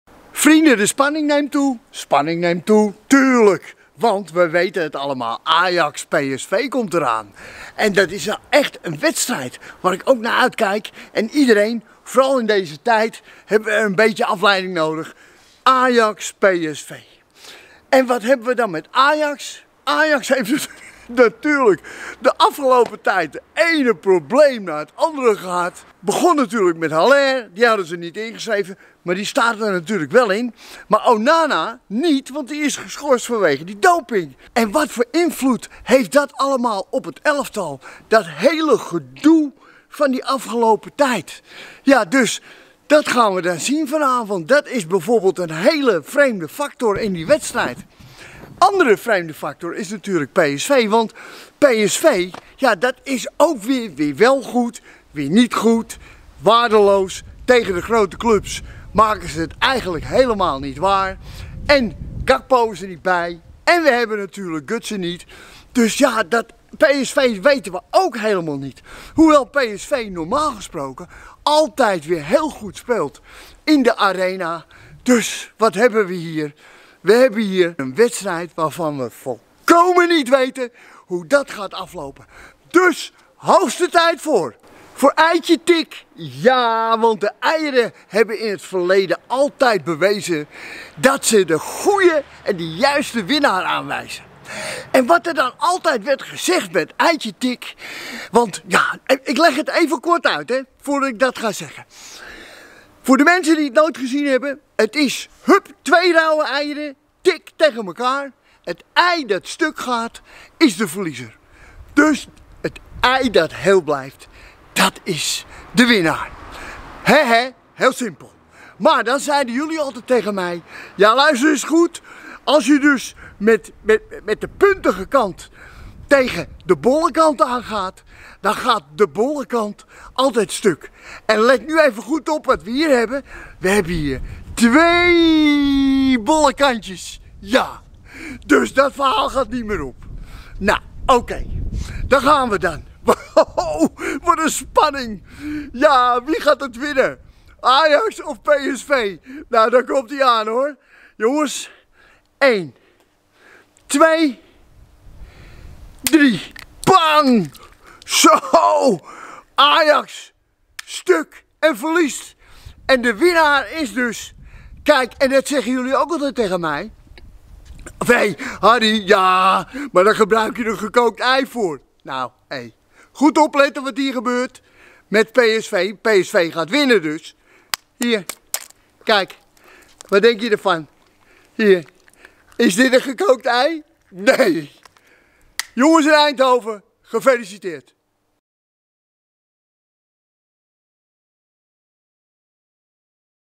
Eitje tik Ajax PSV!
Eitje tik Ajax-PSV! De eieren bepalen de winnaar van deze bekerwedstrijd in de Johan Cruijff Arena!